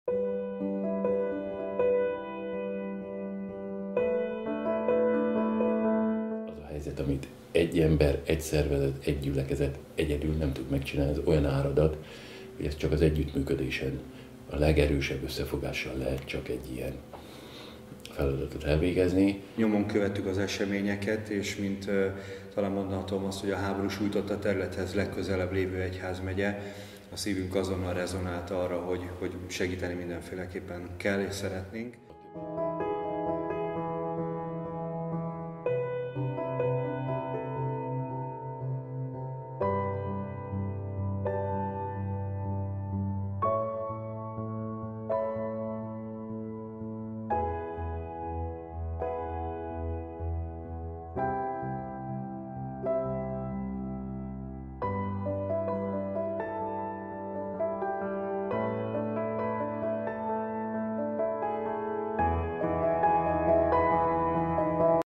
1% LÉLEKKEL, ISMERETTEL – segítség a bajbajutottaknak
„Ez az a helyzet, amit egy ember, egy szervezet, egy gyülekezet egyedül nem tud megoldani.”
Most az 1+1%-oddal is segíthetsz a bajbajutottaknak; kérjük, támogasd adód 1+1%-ával az evangélikus egyházat és az evangélikus alapítványokat!

Magyarországi Evangélikus Egyház | Technikai szám: 0035 | evangelikus.hu/szja-1